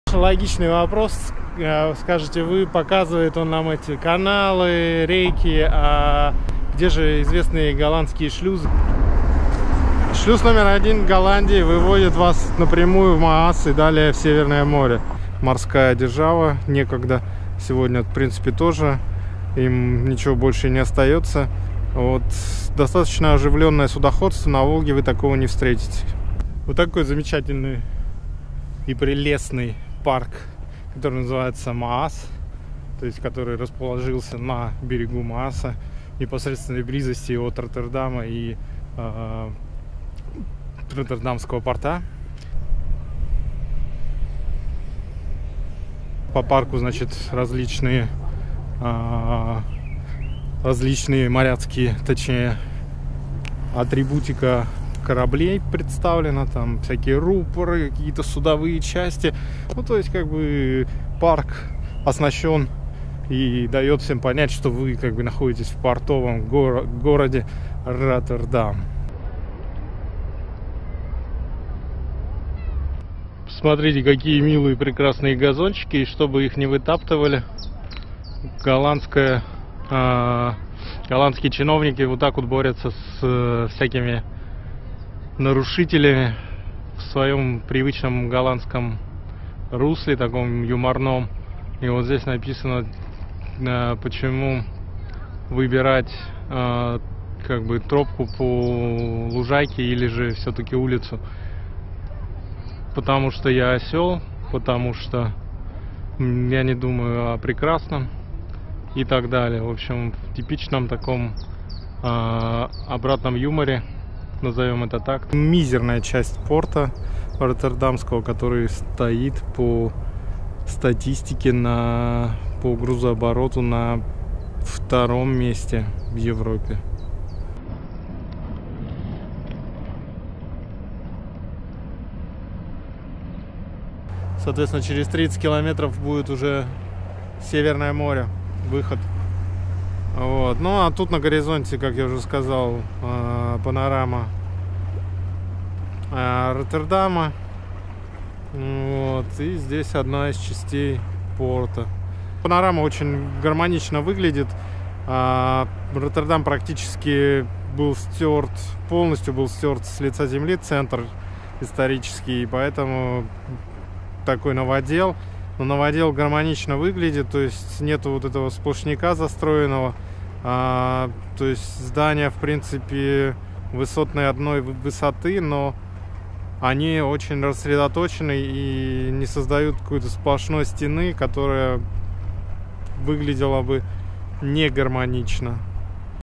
Голландские записки, часть 2 / Парк Маас и скайлайн Роттердама
Прогулка по парку Маас на свежем воздухе и панорама Роттердама.